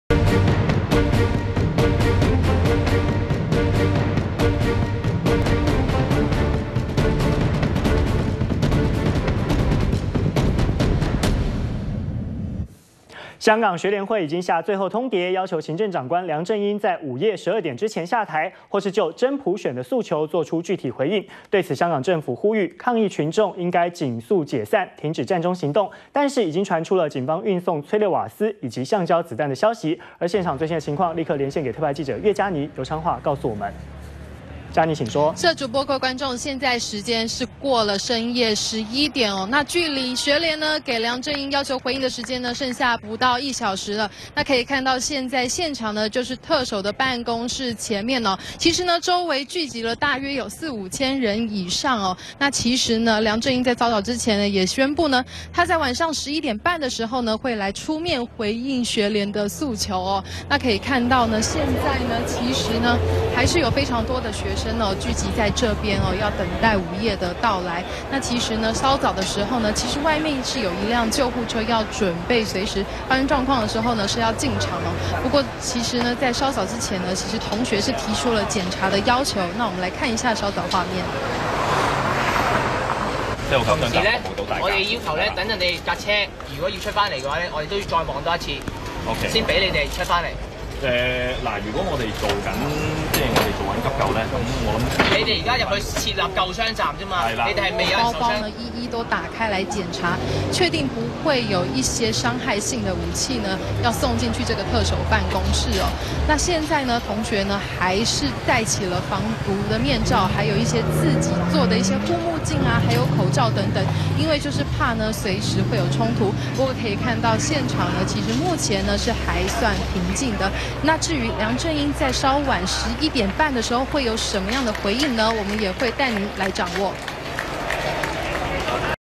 【2014.10.02】學聯通牒倒數計時 港府呼籲理性 -udn tv
香港學聯會已經下最後通牒，要求行政長官梁振英在午夜十二點之前下台，或是就真普選做出具體回應。對此香港政府呼籲抗議群眾儘快解散，停止佔中行動。但是已經傳出，警方運送催淚瓦斯和橡膠子彈的消息，現場最新情況，立刻連線特派記者樂嘉妮、游昌樺告訴我們。完整內容 , FB , Twitter , G+